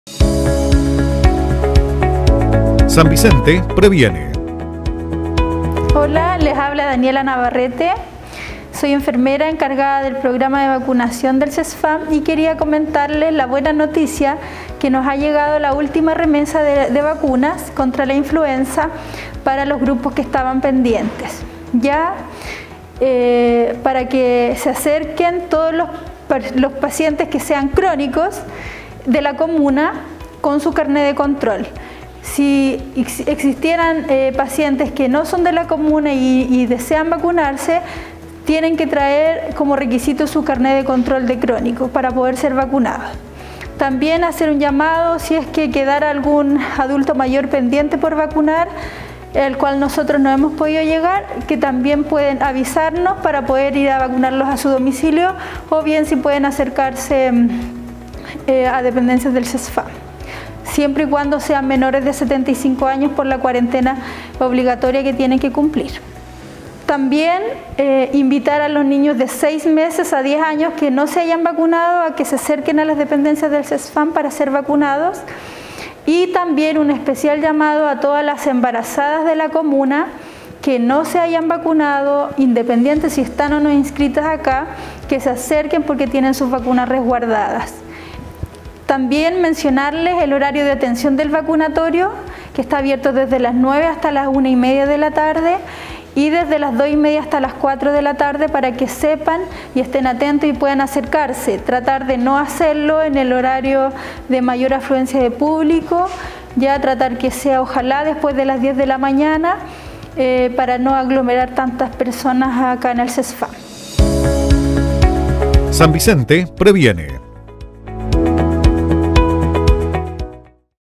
VACUNACIÓN CONTRA INFLUENZA PARA ENFERMOS CRÓNICOS
La encargada del Programa de Vacunación del Cesfam, Daniela Navarrete, informa a la comunidad que se cuenta con una nueva remesa de vacunas contra la influenza, la que será destinada, principalmente, a pacientes crónicos que aún no han sido inmunizados, por lo que éstos deben acercarse al establecimiento con su carné de control. Aquellas personas que presenten patologías crónicas y no están inscritos en el Cesfam de San Vicente, también pueden hacerlo, presentando el mismo documento.

No obstante, agrega que también podrán acceder a esta vacuna adultos mayores que aún no la han recibido. Para ello, deben llamar al Cesfam para ir a vacunarlos a su domicilio o llegar hasta el Cesfam, sólo si son menores de 75 años, ya que este grupo se encuentra en cuarentena obligatoria.

De la misma manera, deben hacer los padres de niños y niñas, entre los seis meses y los 10 años, que no fueron vacunados aún y las embarazadas, a quienes la profesional formuló un especial llamado.

El horario de atención del vacunatorio es de 9:00 a 13:30 horas en la mañana, y desde las 14:30 a las 16 horas, en la tarde.